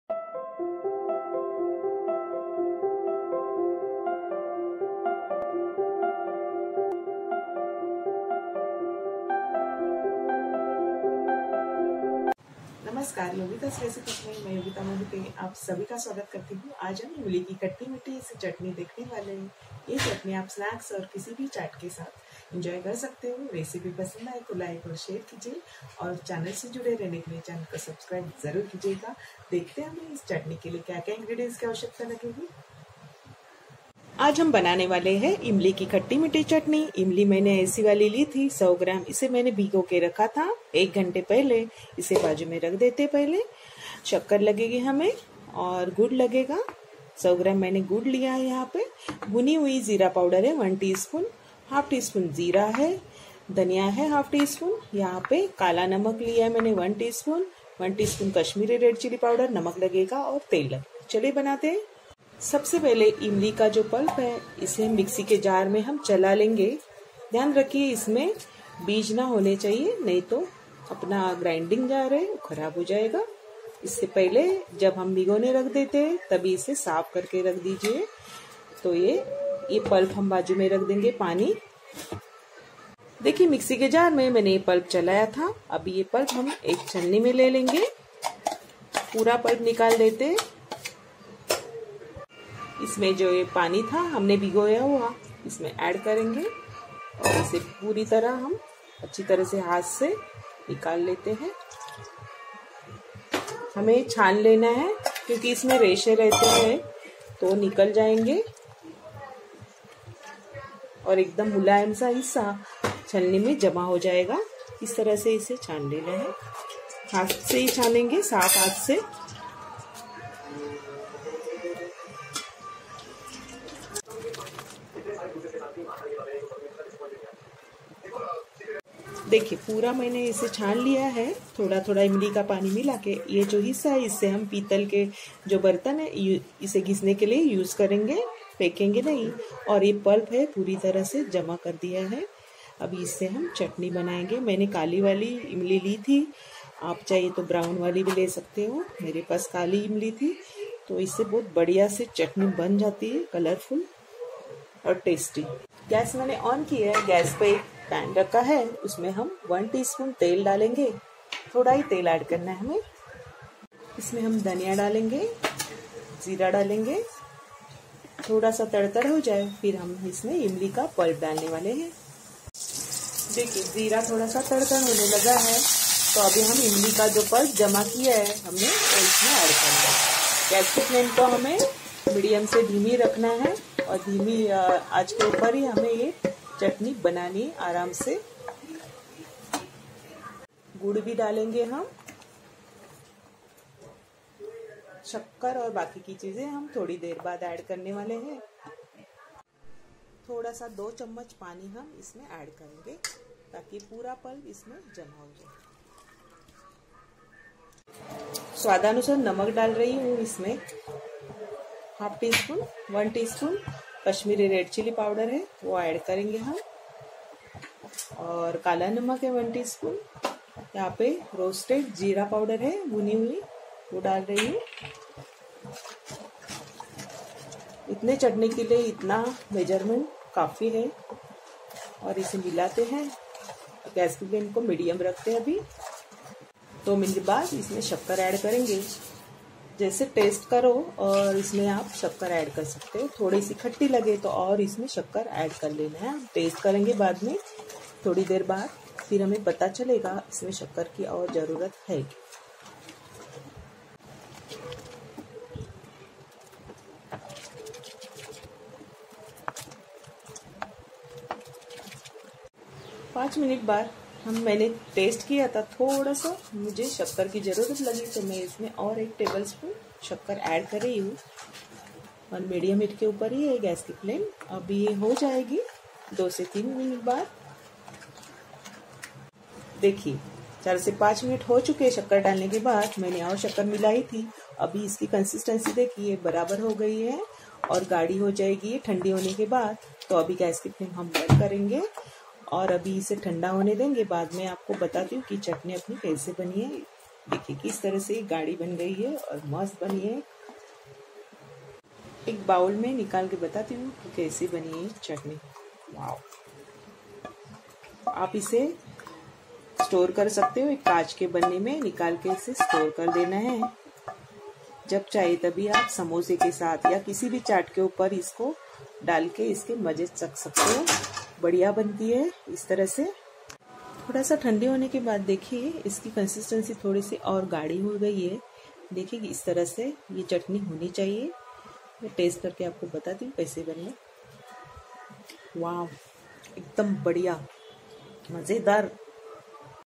इस आसान तरीके से बनाएं इमली की खट्टी मीठी चटनी|Imli ki Chatni| Tamrind Chatni 😋
Ingredients:

100 Gram :Tamrind
100 Gram: Jaggery
1/2 Cup: Suger
1 Tsp: Roasted Cumin powder
1 Tsp: Kashmiri Mirch powder
1 Tsp: Black Salt
1 Tsp: Salt
1 Tsp: Cooking Oil
1/2 Tsp: Coriander Seeds
1/2 Tsp: Cumin Seeds